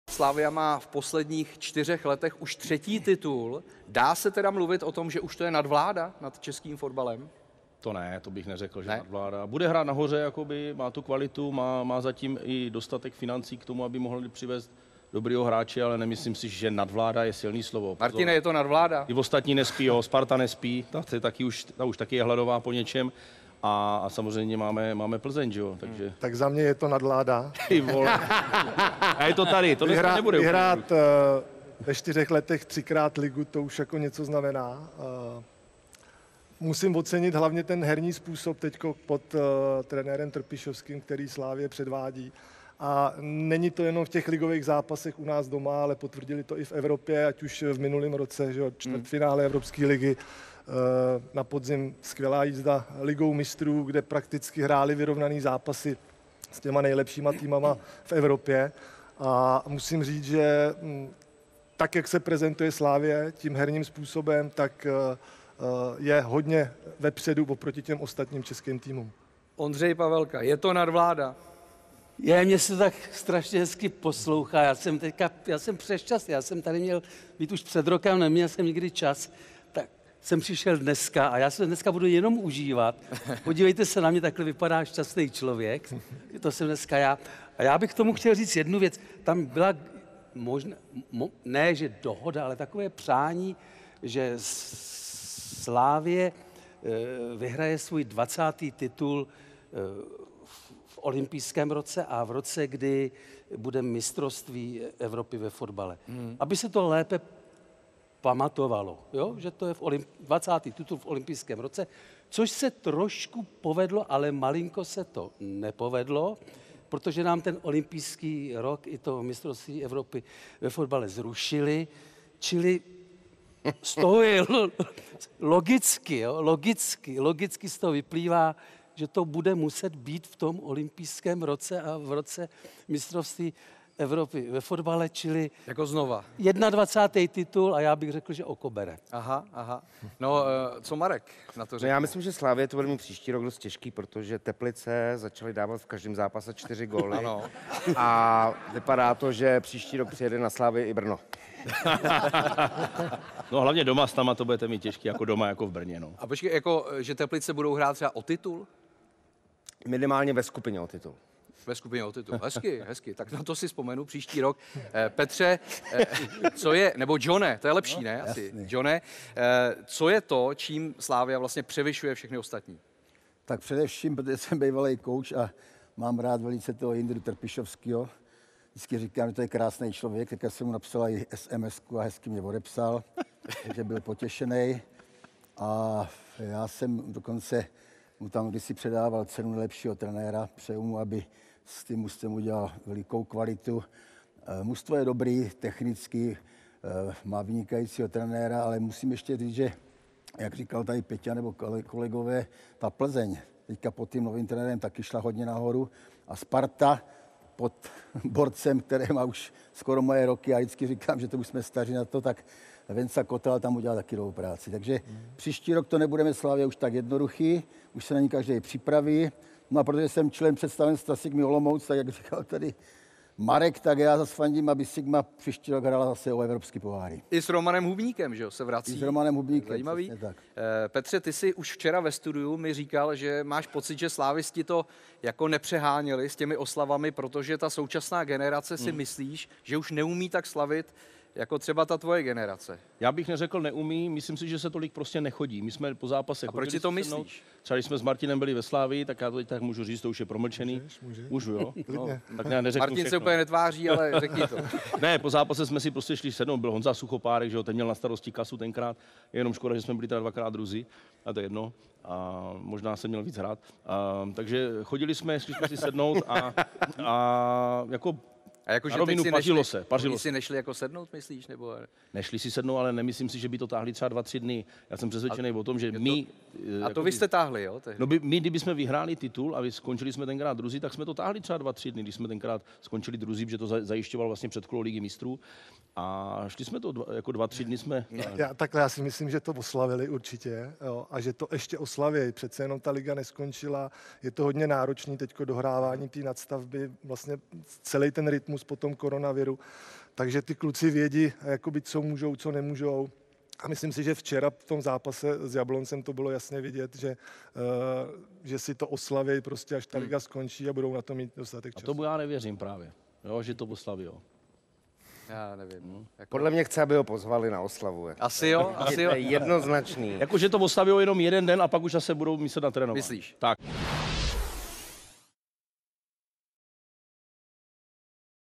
TIKI-TAKA: Slavia slaví třetí titul za čtyři roky. Je to nadvláda?
Slavia mohla po výhře nad Plzní s předstihem slavit obhajobu mistrovského titulu. Mají sešívaní nadvládu nad českým fotbalem? A umí dnešní fotbalisté slavit tak jako Petr Švancara, tedy několik dní v kuse?